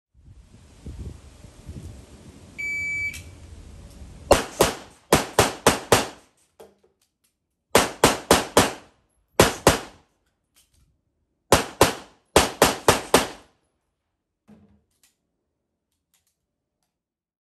IPSC Handgun Training 19092021 Speed Shoot
IPSC Handgun Training 19092021
CZ Shadow 2 Prod Optic
with CZ Shadow 2 EemannTech Upgrade Kit
Shield RMS 2
AST Roissy